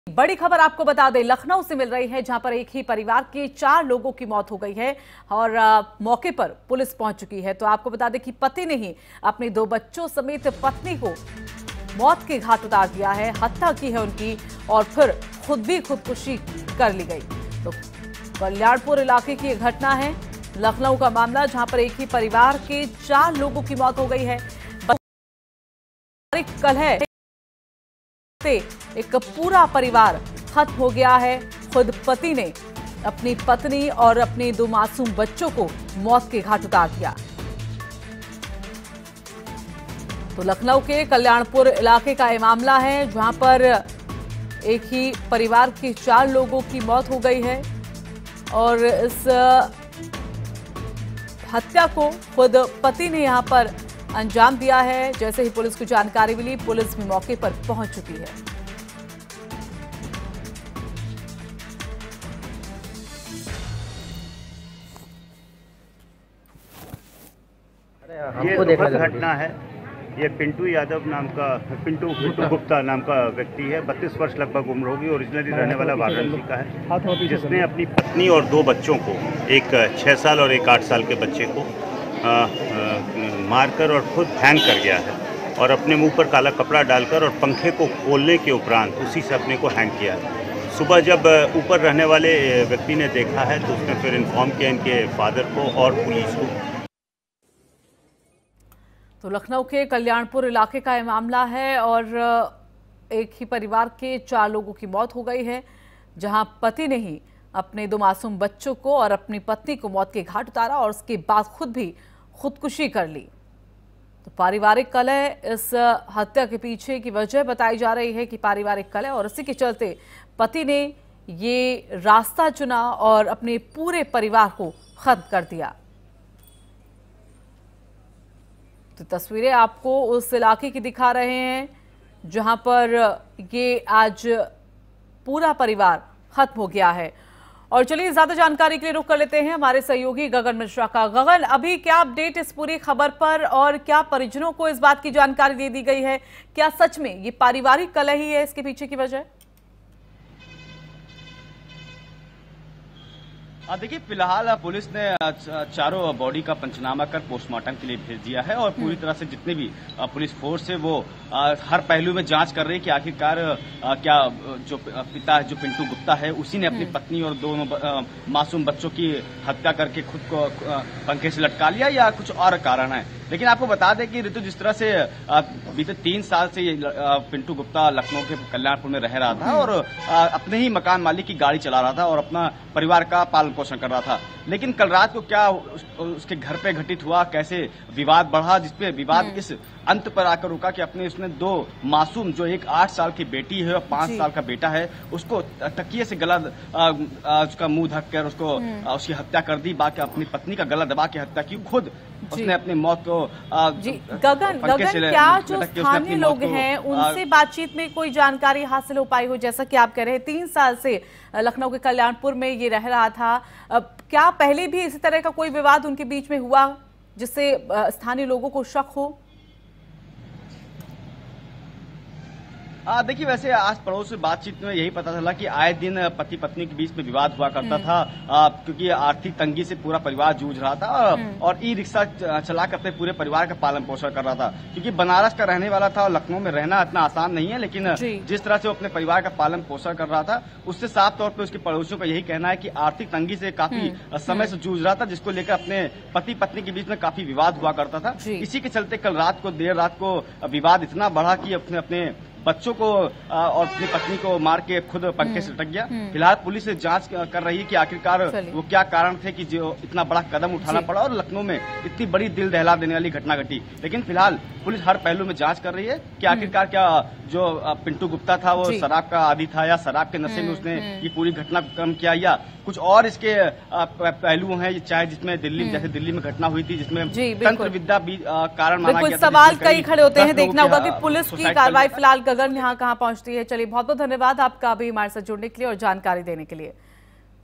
लखनऊ : दो बच्चों और पत्नी की हत्या के बाद पति ने की आत्म हत्या
एक परिवार के चार लोगों की मौत...पति ने ही अपने दो बच्चों और पत्नी की हत्या कर खुद की खुदकुशी की...कल्याणपुर इलाके की है घटना